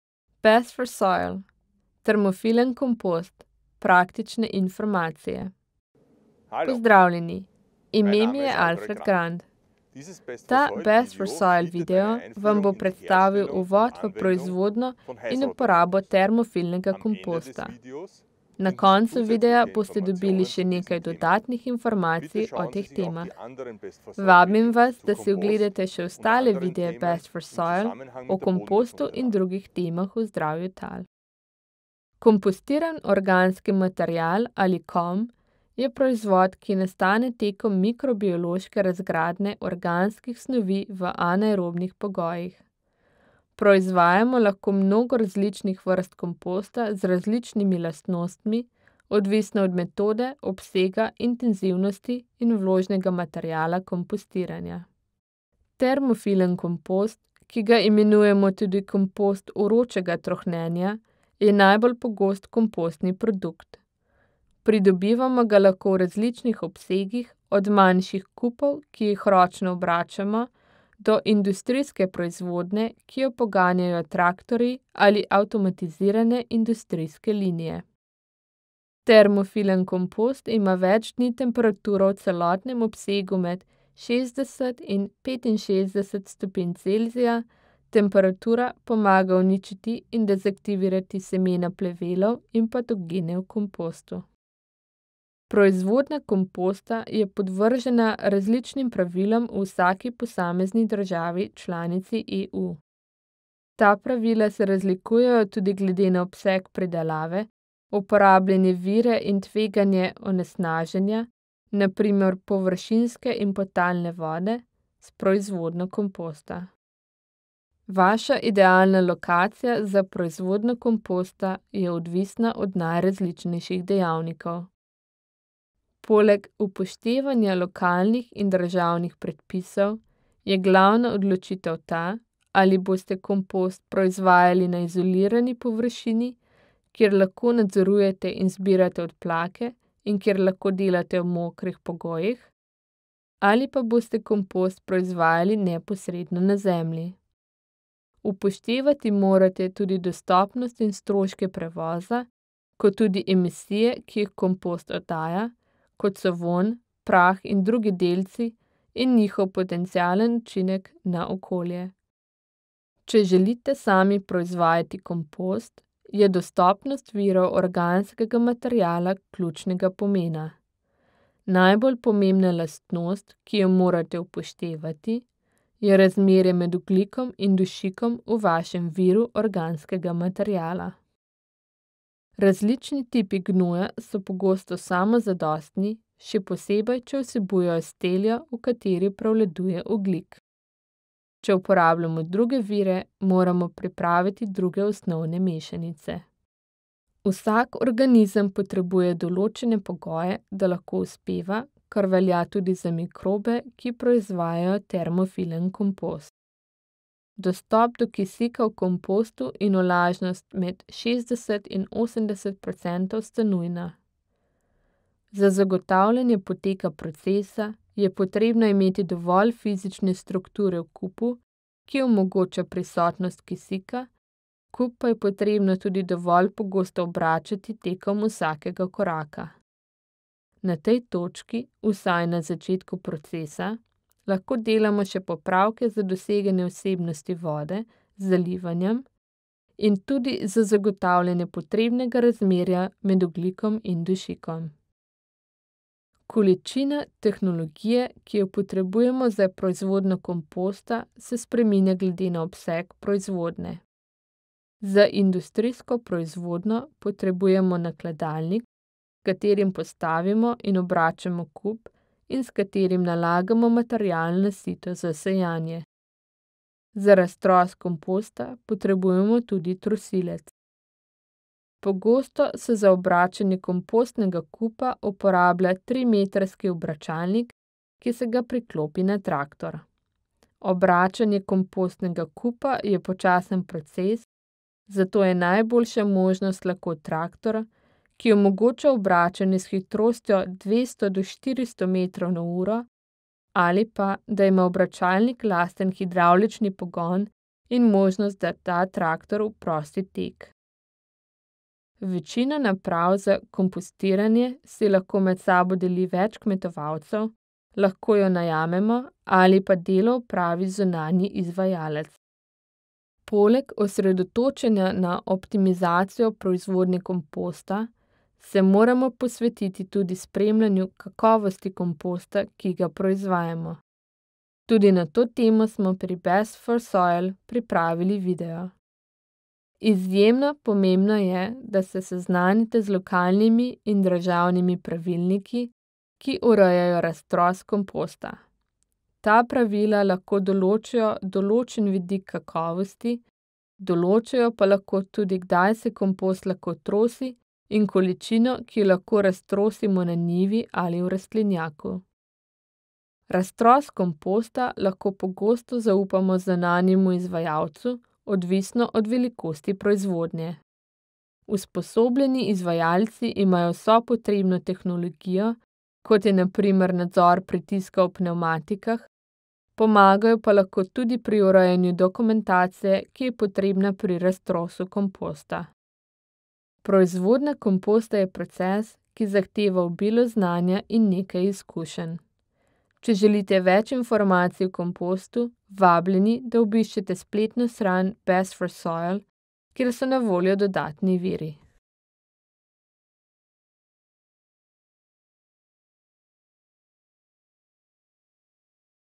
Termofilen kompost – praktične informacije
Ta video Best4Soil vam poda uvod v proizvodnjo in uporabo termofilnega komposta. Na kocu videa pa je še nekaj dodatnih informacij o teh temah.